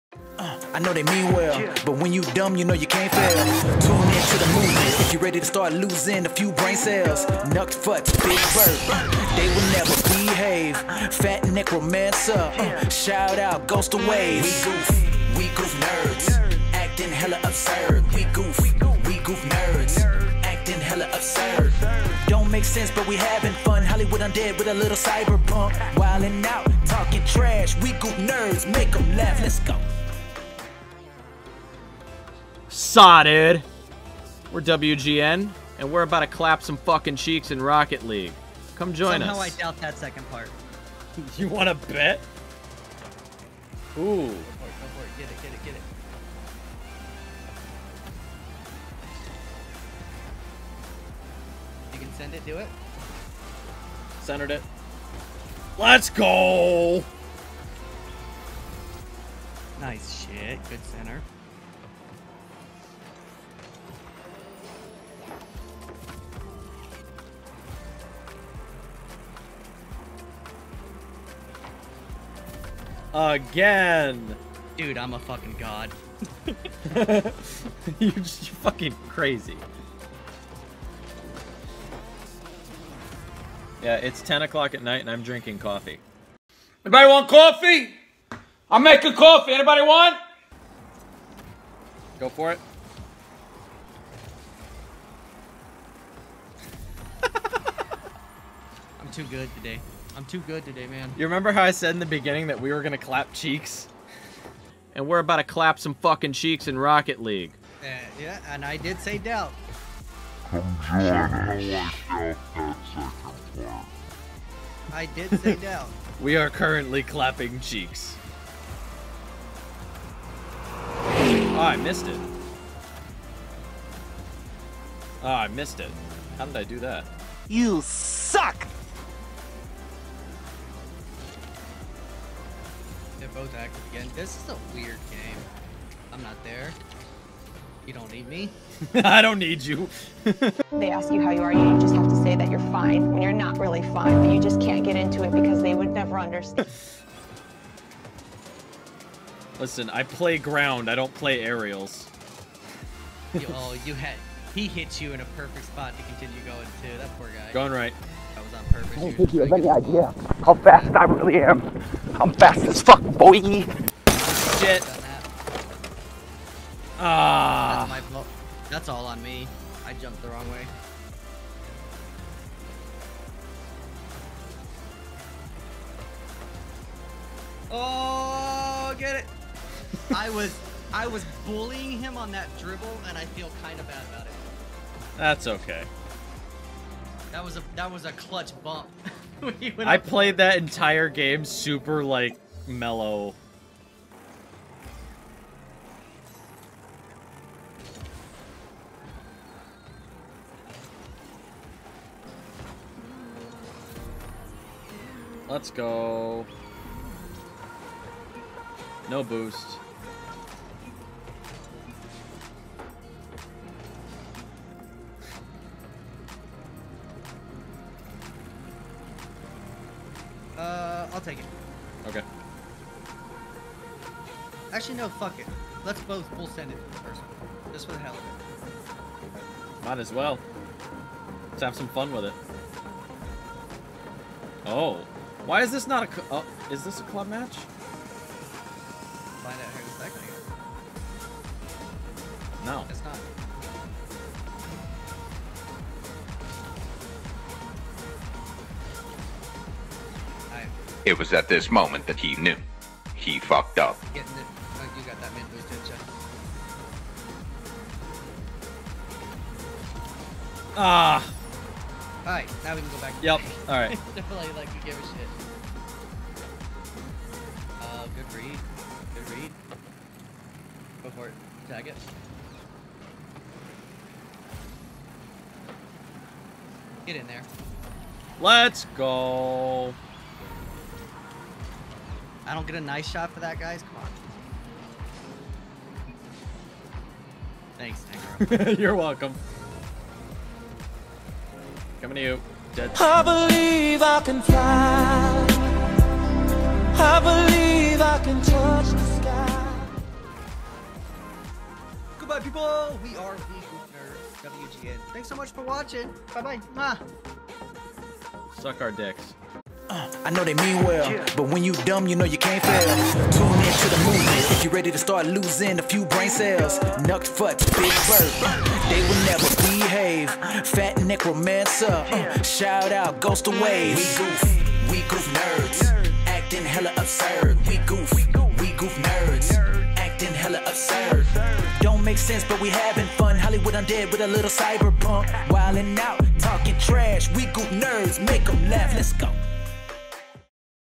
A Gold and a Plat Popped Into Casuals.... YOU WON'T BELIEVE WHAT HAPPENED!
Alright, so the title may be a bit misleading. We are ass. We know that. But we had some fun, and got some decent goals. Hope you enjoy!